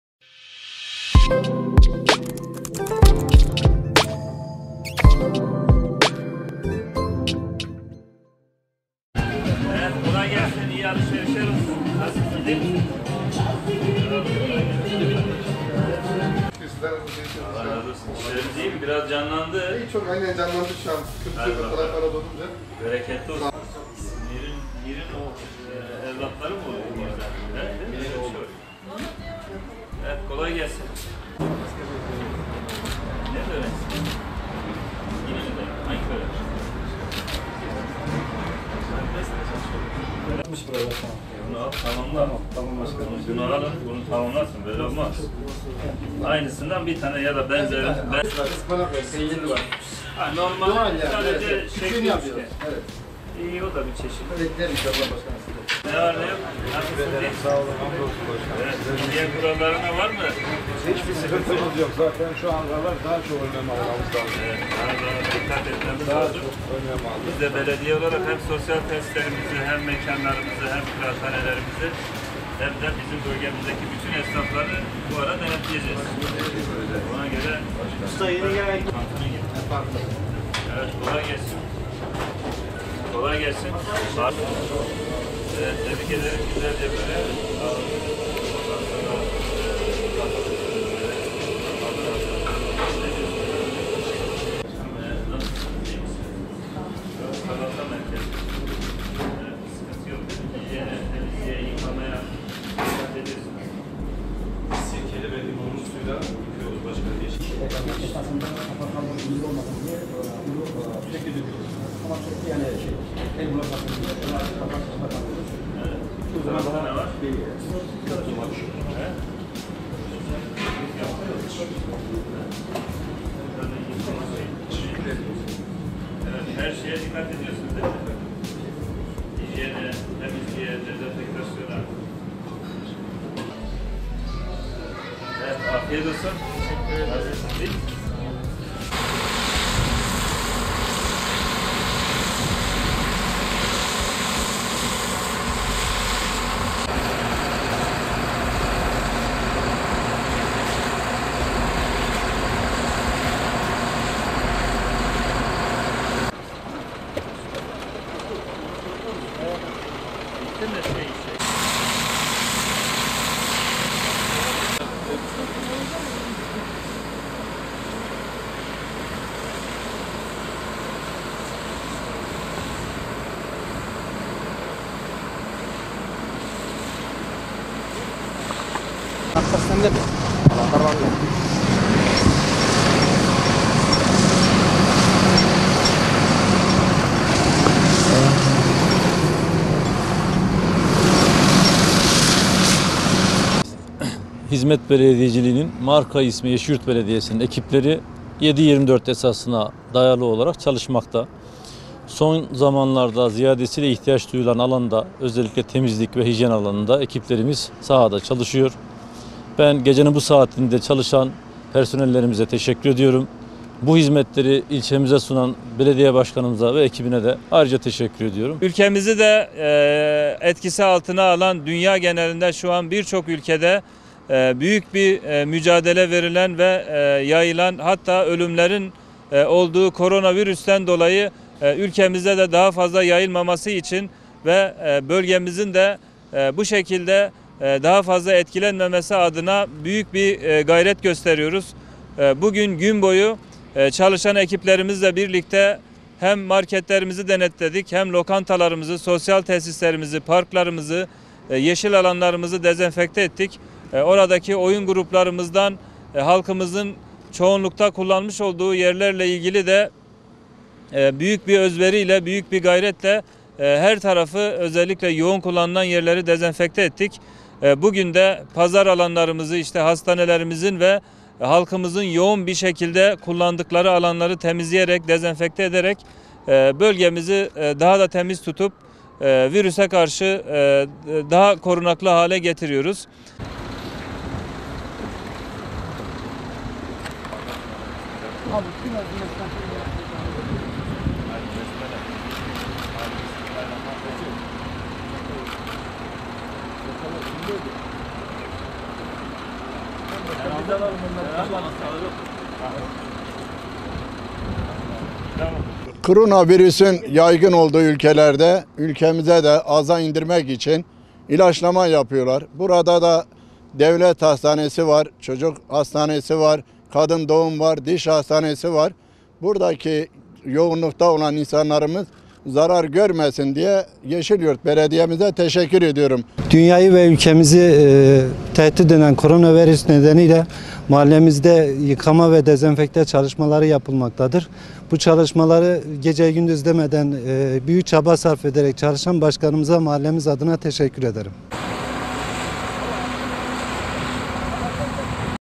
Yeşilyurt’ta ‘korona virüs önlemleri arttırıldı
Malatya’nın Yeşilyurt İlçesi Belediye Başkanı Mehmet Çınar, dünyada hızla yayılan ‘korona virüs’ salgını nedeniyle ilçede ilçede hizmet veren fırın, lokanta ve manavları denetledi. İlçedeki yollar ise sabunlu su ile yıkandı.